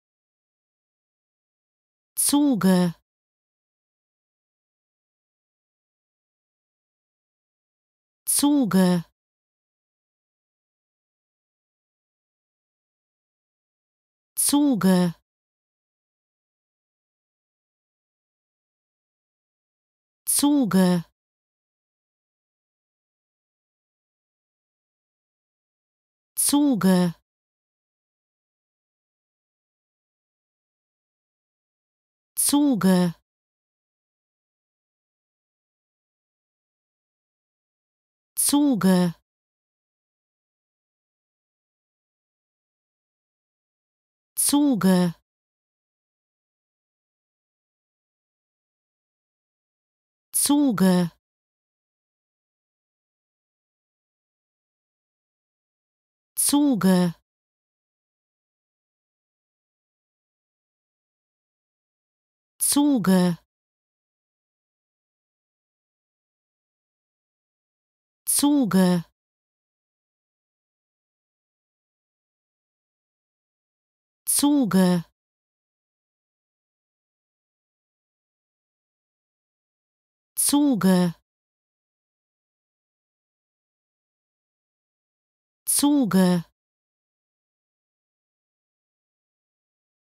How to Pronounce ''Zuge'' (Turn) Correctly in German
Learn how to say ''Zuge'' in German with this short pronunciation tutorial.